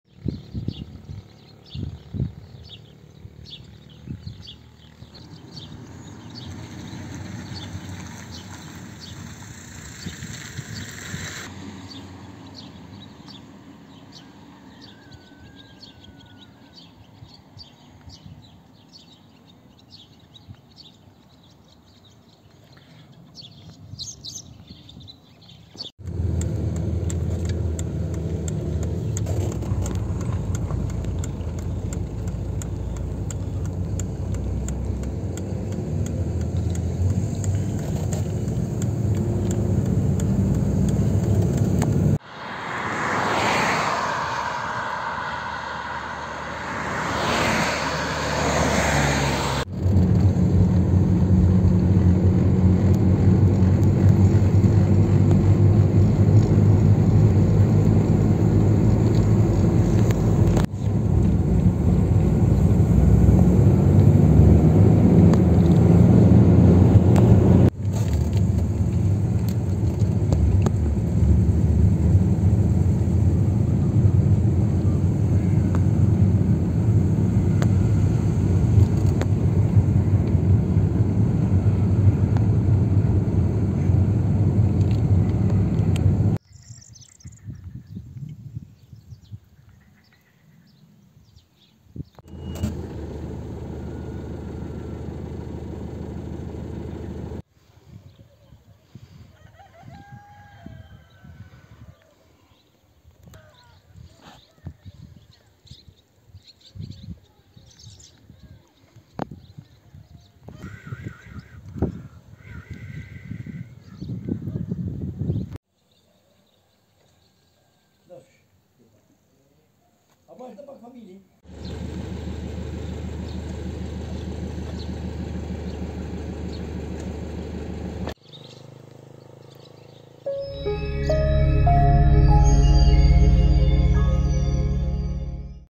Vrasja e Ardian Nikulajt në Shëngjin, autori furnizoi motorin me karburant në aksin Lezhë-Shkodër!
reportTV lajmeshqip lajmeMotori i përdorur nga autori i vrasjes së biznesmenit Ardian Nikulaj në Shëngjin është furnizuar në një pike karburanti ne superstraden Lezhë-Shkoder. Kjo është zbuluar pas kqyrjes së kamerave të sigurisë dhe policia ka shoqëruar pyetur punonjësin e karburantit i cili ka komunikuar me drejtuesin e motorit.